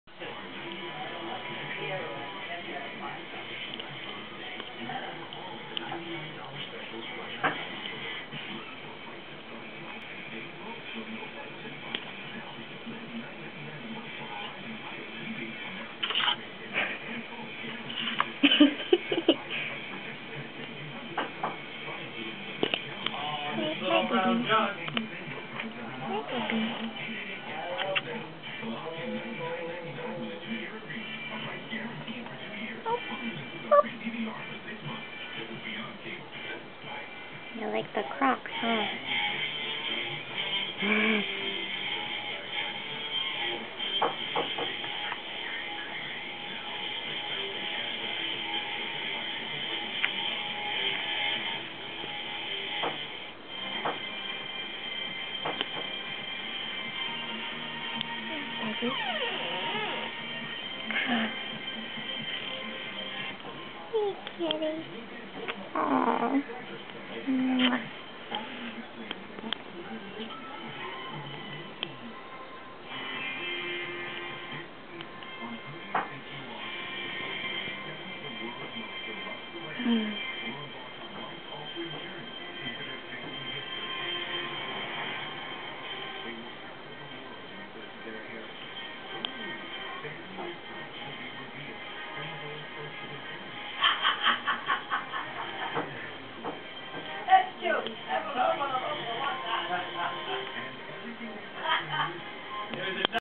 charlie and his siblings at about 3 weeks
i was obviously interested in the striped one then, but i'm glad i picked charlie :o)